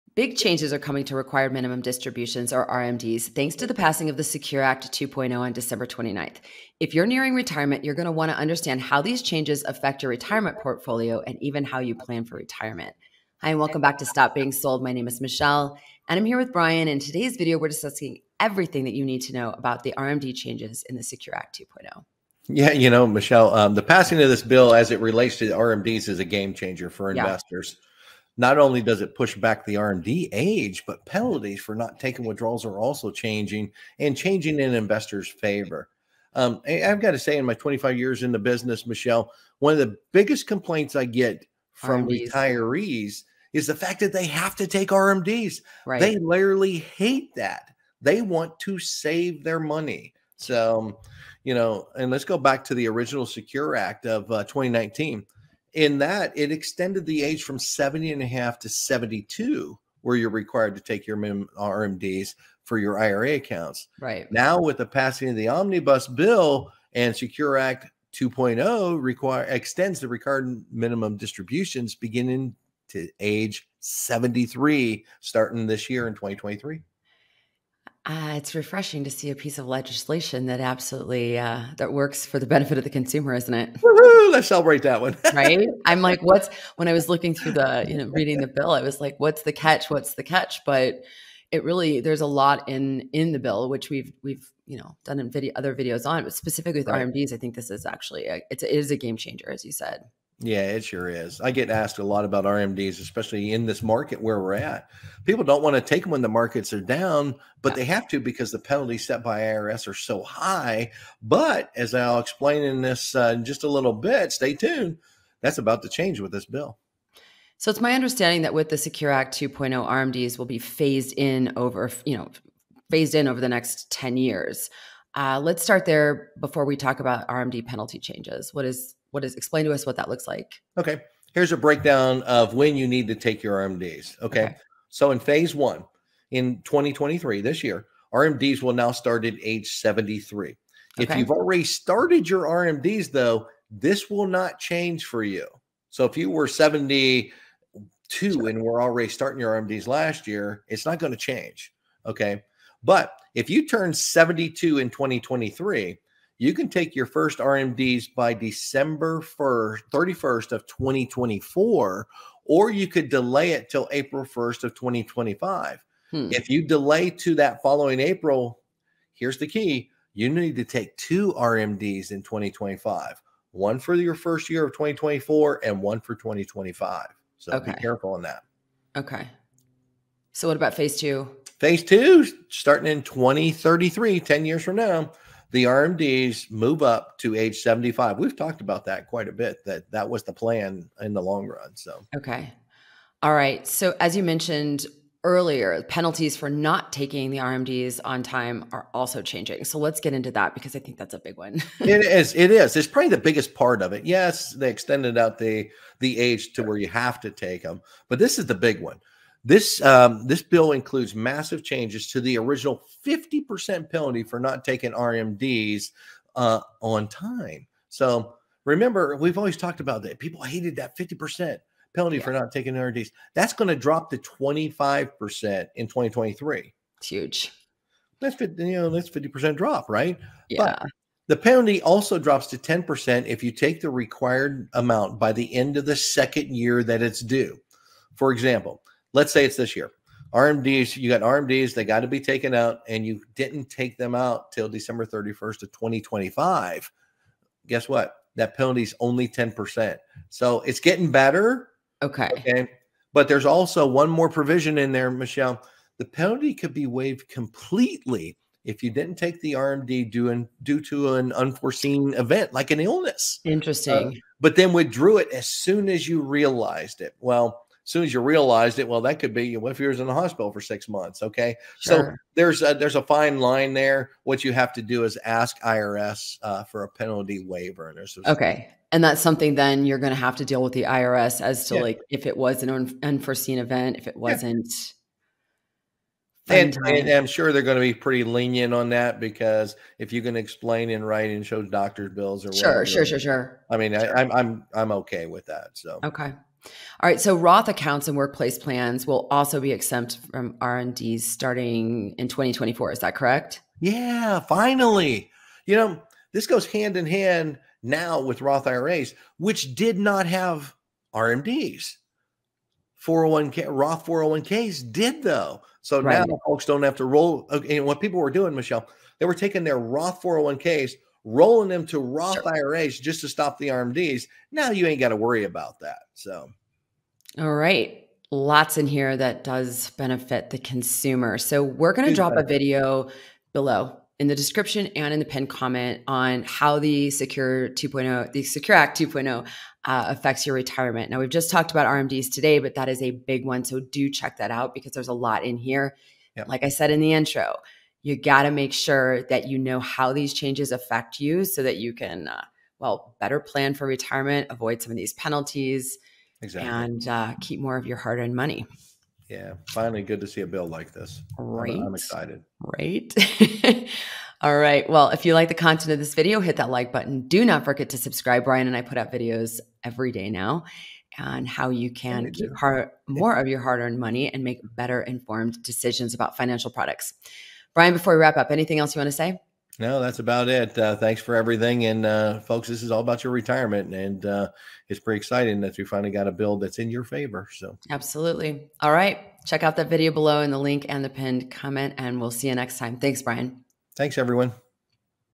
Required Minimum Distribution Changes for 2023
The original SECURE Act of 2019 extended the age from 70 ½ to 72 where you are required to take Required Minimum Distributions (RMDs) from your IRA accounts.

Now with the passing of the Omnibus Bill, SECURE Act 2.0 extends Required Minimum Distributions (RMDs) beginning date to age 73, starting in 2023.

If you are nearing retirement, you need to stay up to date on these changes, and how they may affect your retirement portfolio. Pay attention to the details including how the penalties for not taking withdrawals on time will affect you…

With the passage of SECURE Act 2.0, Required Minimum Distributions (RMDs) are phased in over the next 10 years. Here’s the breakdown of when you need to take RMDs:

In phase #1: RMDs will now start at age 73, starting in 2023. If you have already started RMDs, this will not change for you.

If you turn 72 in 2023, you can take your first RMD by December 31st, 2024, or you could delay it to April 1st of 2025. If you delay to April, you will need to take 2 RMDs in 2025. One for 2024, and one for 2025.
In phase #2: Starting in 2033, the RMDs move up to 75.

Also included in the bill are massive changes to the original 50% penalty for not taking Required Minimum Distributions (RMDs) on time:

The hefty 50% penalty for not taking RMDs will drop to 25% in 2023.
The penalty drops to 10% if you take the required amount by the end of the second year that it was due. For example: If your RMDs for 2023 aren’t taken, you have till December 31st of 2025 to take the withdrawal and only pay the 10% penalty.
The penalty could be waived completely if you didn’t take the RMD due to an unforeseen event (like illness), but then withdrew it as soon as you could. You would have to ask the IRS for a penalty waiver.

Roth accounts in workplace plans will be exempt from RMDs starting in 2024.